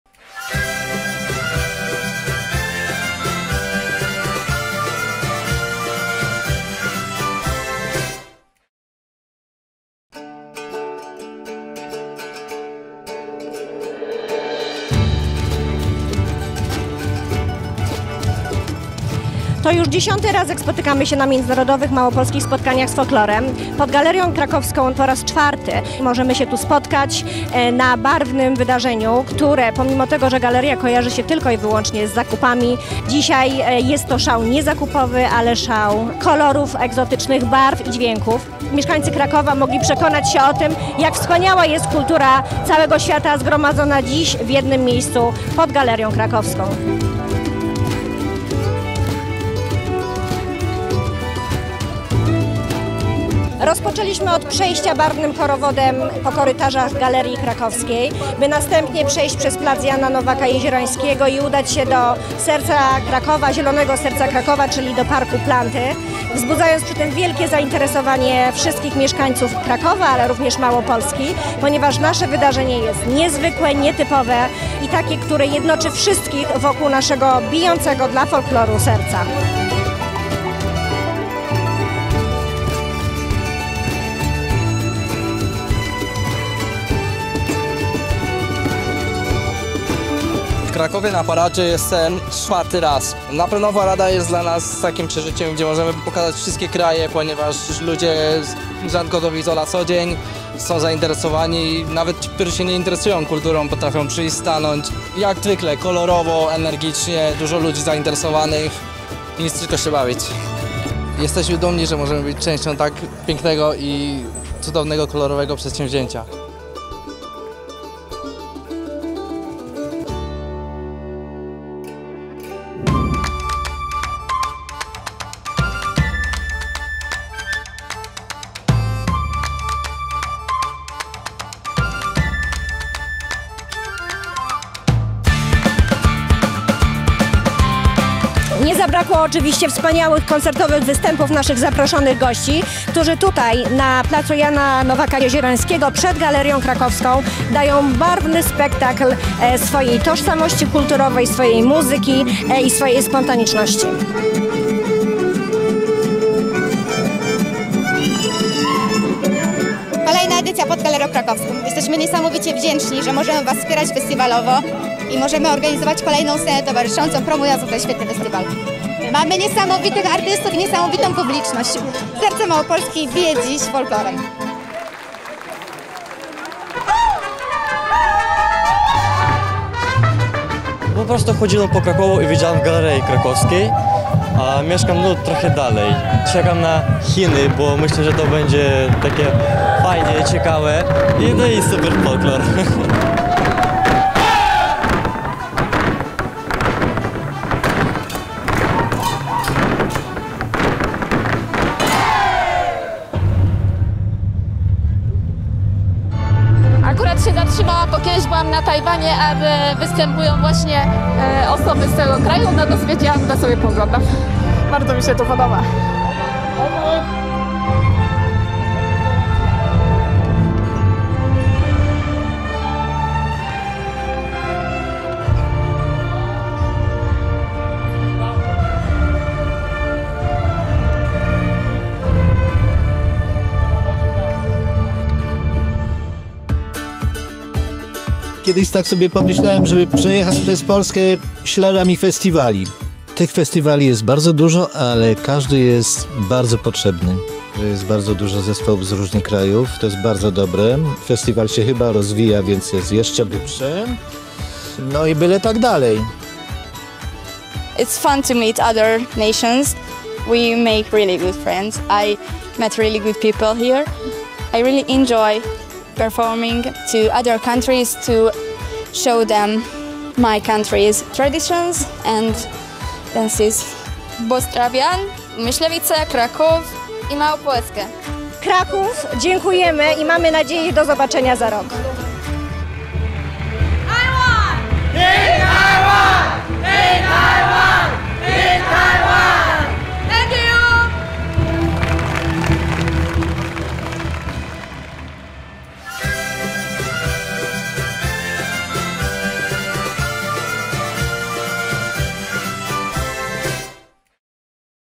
Małopolska Festival: Koncerty na scenie przy Galerii Krakowskiej- Kraków 2024
Od czterech lat Międzynarodowym Małopolskim Spotkaniom z Folklorem towarzyszy scena zlokalizowana w  Krakowie, a dokładniej przy Galerii Krakowskiej. W wyznaczonym dniu festiwaliu, artyści prezentują tam swój bogaty repertuar, zapraszając jednocześnie zarówno do Małopolski, jak i Krakowa i Myślenic. Krakowska część festiwalu rozpoczyna się paradą po Galerii Krakowskiej, następnie kolorowy korowód maszeruje na Planty, aby finalnie powrócić na Plac Jana Nowaka Jeziorańskiego i tam częstować publiczność znakomitym folklorem z całego świata.